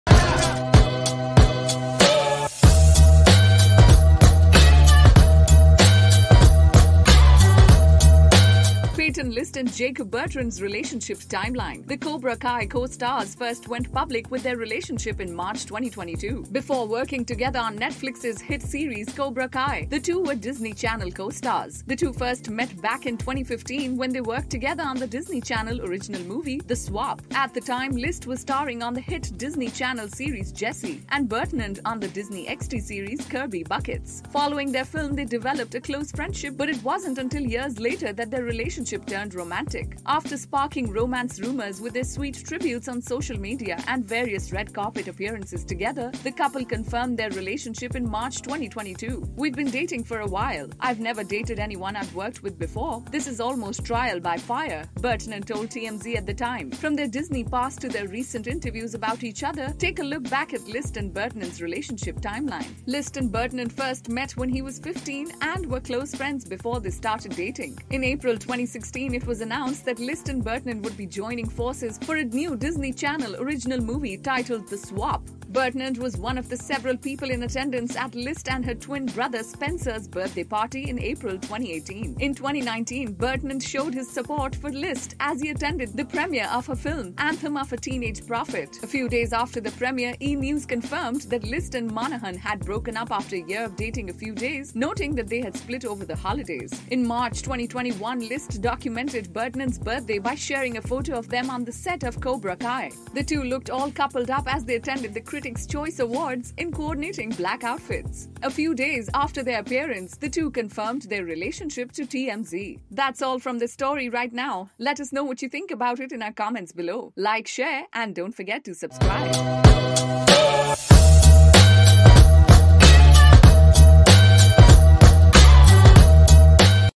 Peyton List and Jacob Bertrand's Relationship Timeline
Peyton List and Jacob Bertrand's relationship has been years in the making Before working together on Netflix's hit series Cobra Kai, the two were Disney Channel costars.